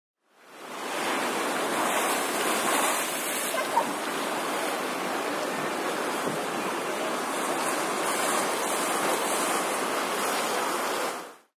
Attention à la ... vague
quand l'attention portée à des kayakistes vous arrose